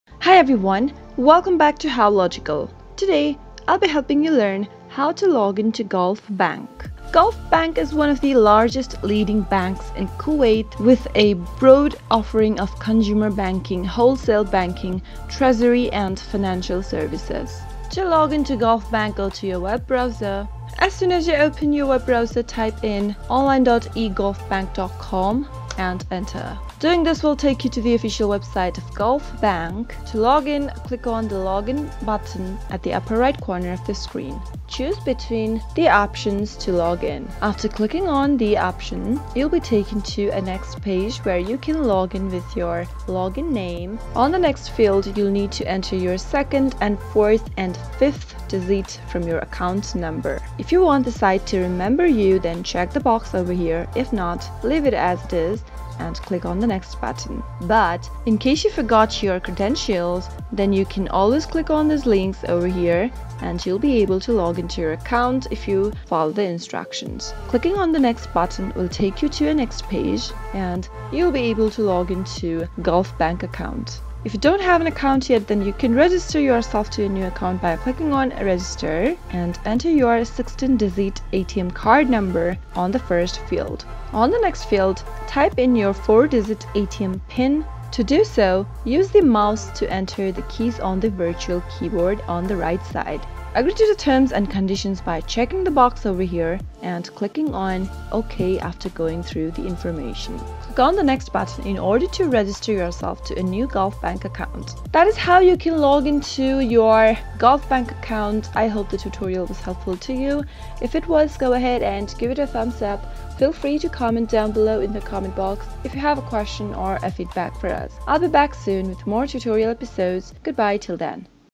Gulf Bank Online Banking Login
Learn how to login to gulfbank online banking account. This video tutorial walks you through the step by step process on how to login to your gulf bank online account. For this, you need to go to the official login page i.e. Here, click on the login button and enter your user ID and password on the appropriate field. Finally, click on the login button.